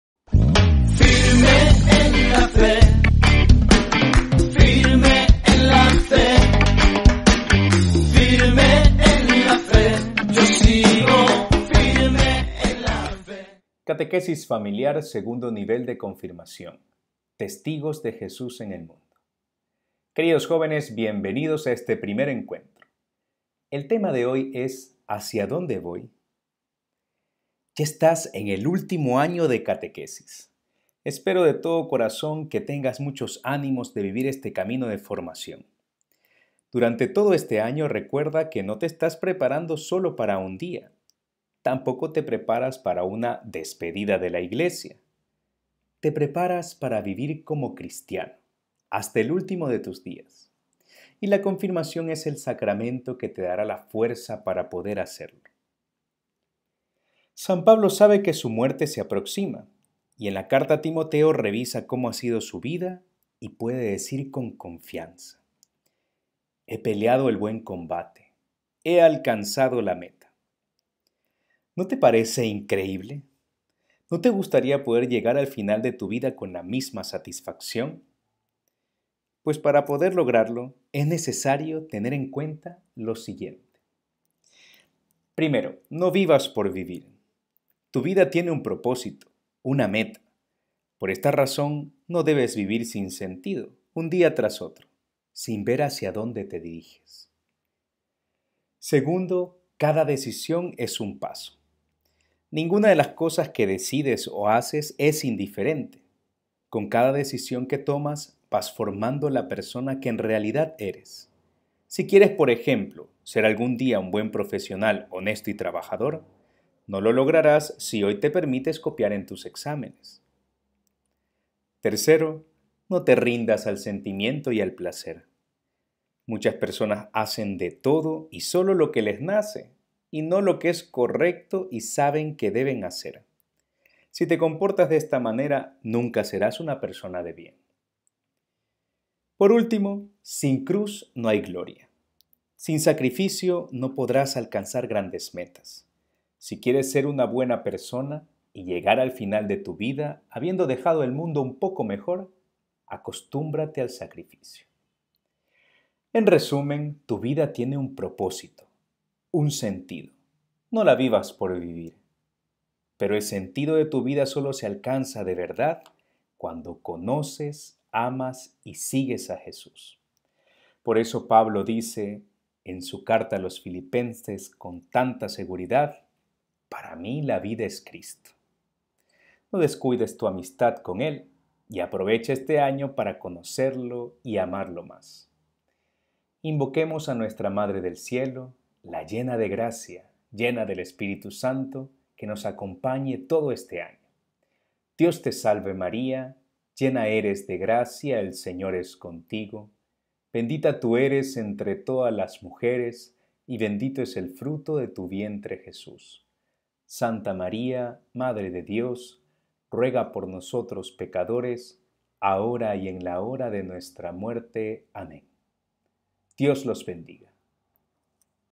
SEGUNDO NIVEL DE CONFIRMACIÓN - TEMA 1: ¿Hacia dónde voy?
Este Segundo nivel, último año -formal- de catequesis, en preparación a nuestra Confirmación, iniciaremos descubriendo hacia dónde vamos  en nuestra vida Cristiana.

CatequesisFamiliar - Un Aporte de la Arquidiócesis de Guayaquil y la Conferencia Episcopal Ecuatoriana (CEE)